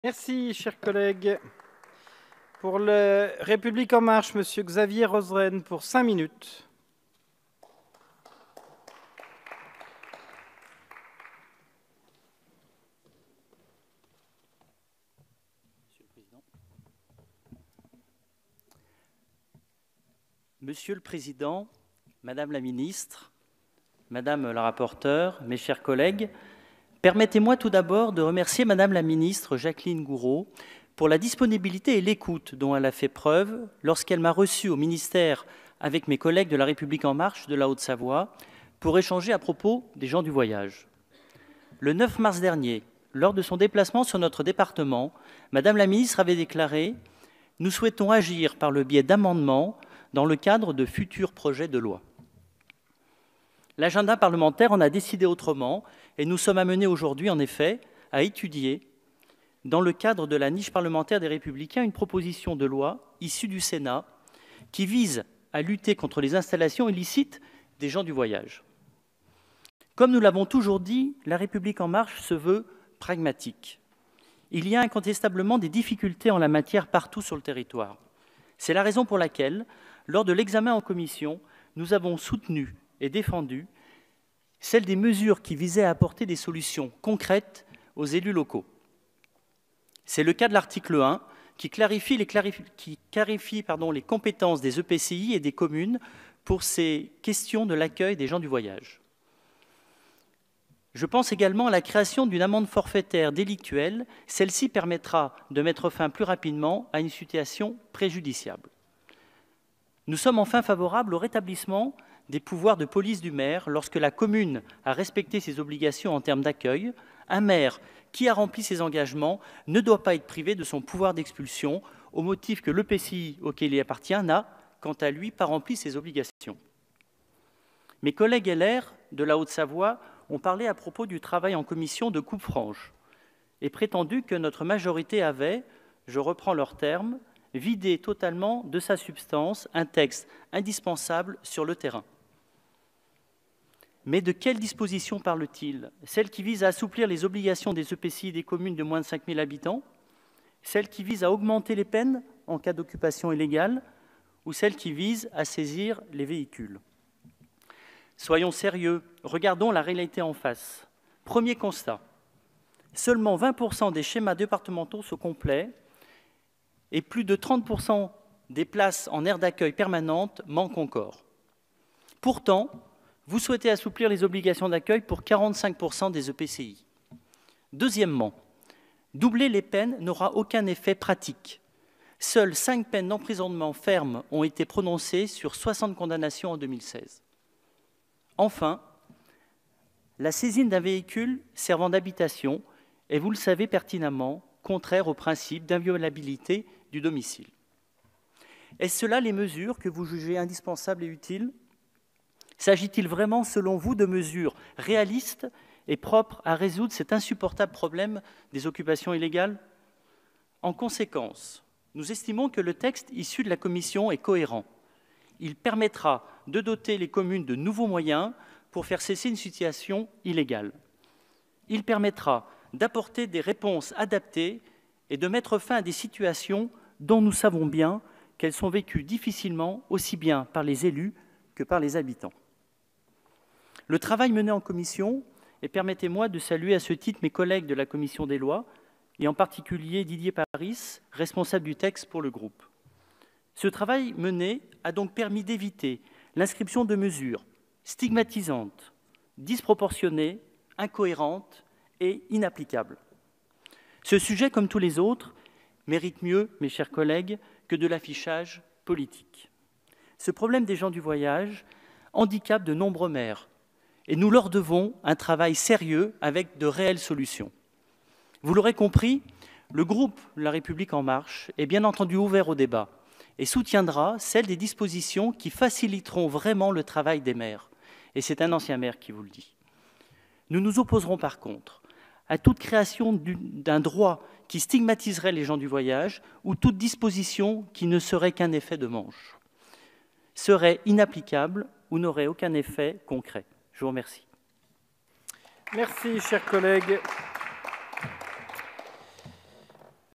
Gens du voyage et installations illicites - Intervention en discussion générale
Intervention en séance publique lors de la discussion générale sur la proposition de loi relative aux gens du voyage et à la lutte contre les installations illicites